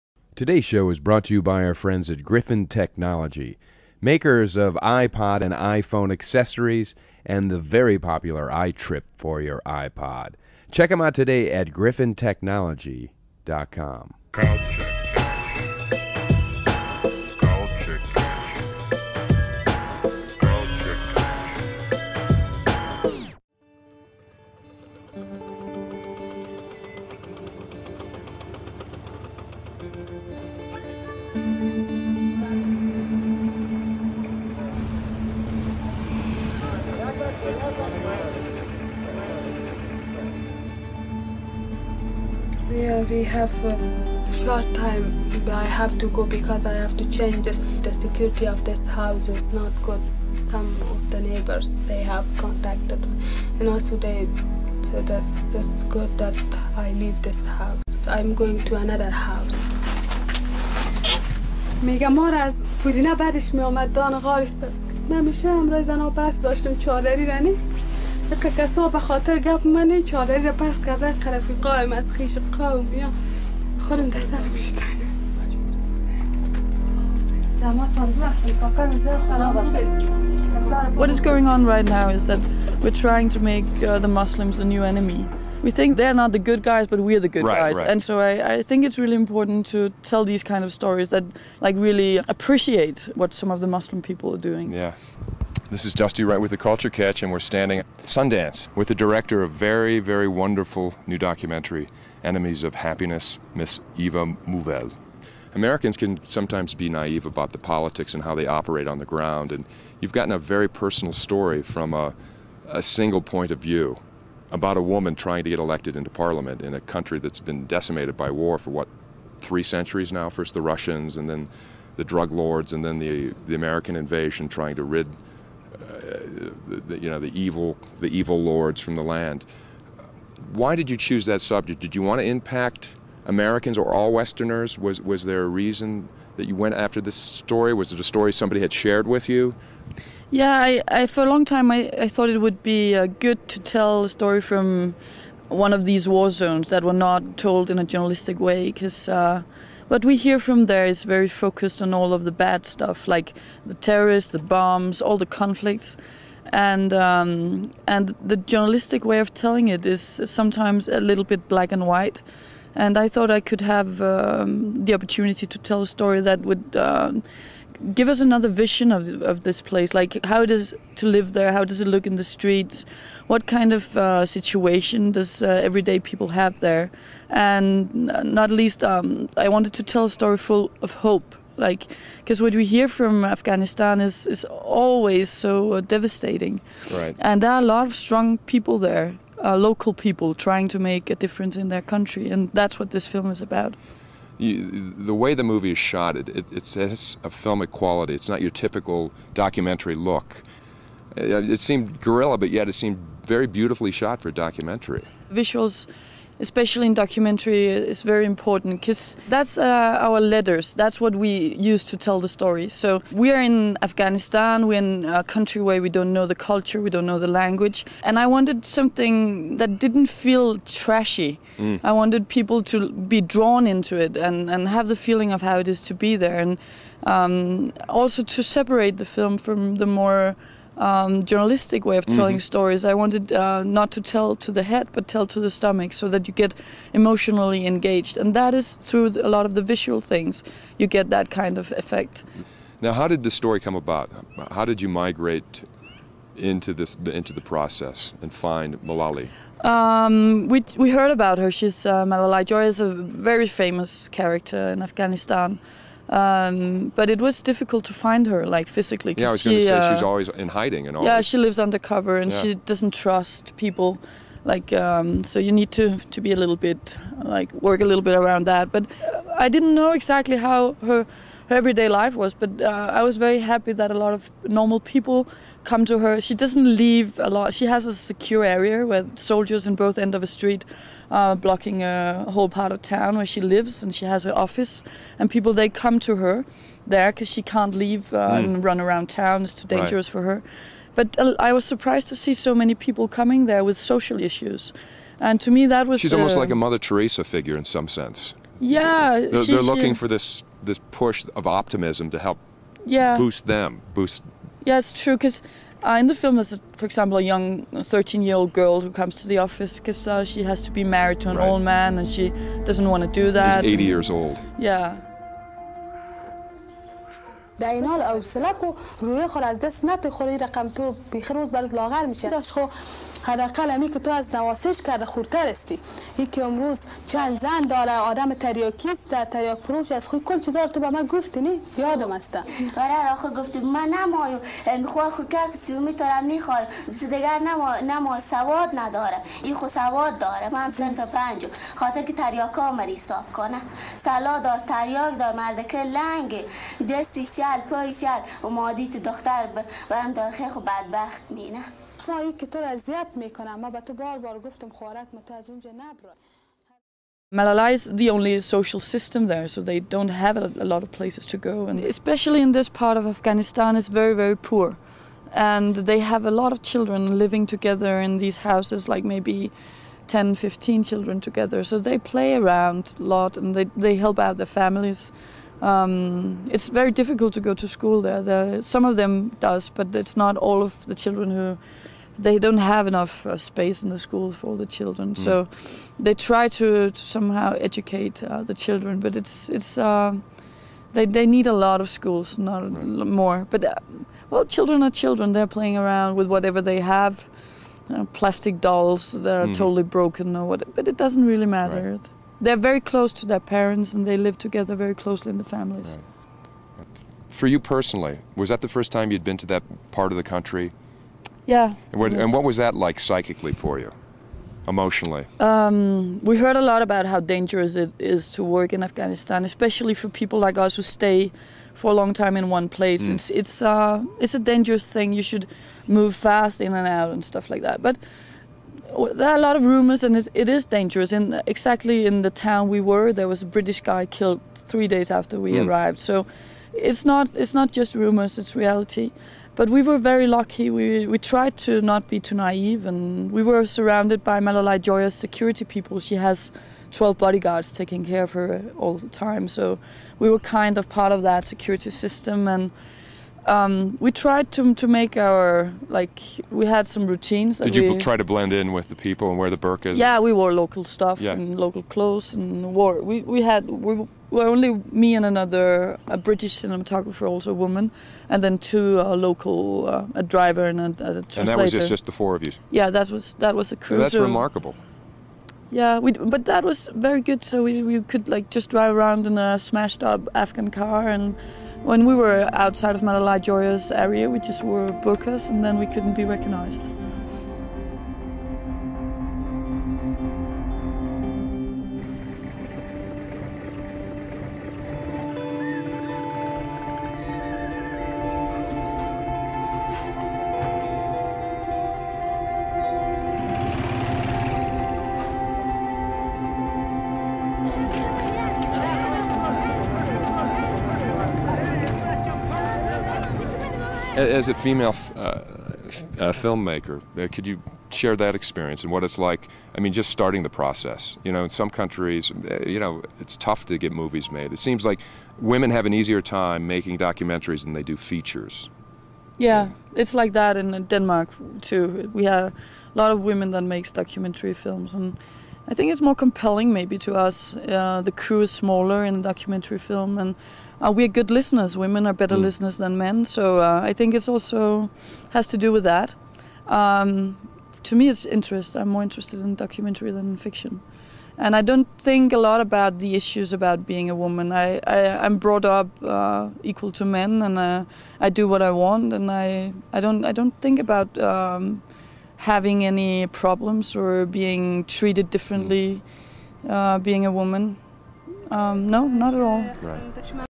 Eva Mulvad on Culture Catch! (Part 1)
Part 1 of 2. Director Eva Mulvad Exposes Afghanistan on Video Podcast!
Listen to Danish director Eva Mulvad share her moving story about a courageous young woman fighting for change in Afghanistan in her award-winning documentary Enemies of Happiness. Her journey will change you forever. (Powered by Griffin.)